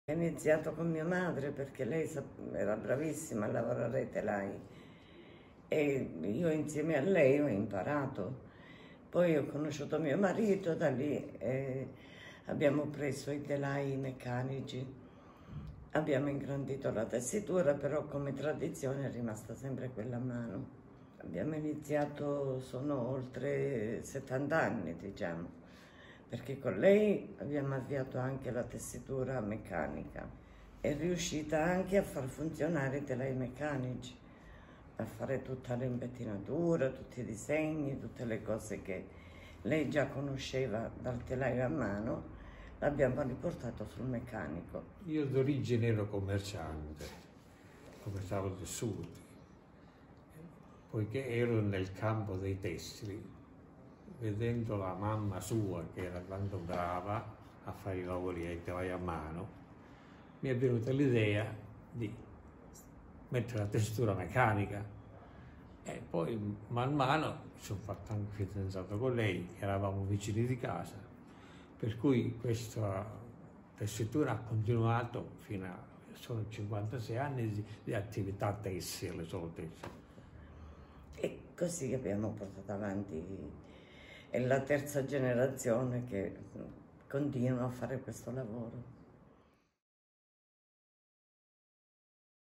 Fabbrica Tessile Bossio
Intervista a Elena e Angelo Bossio -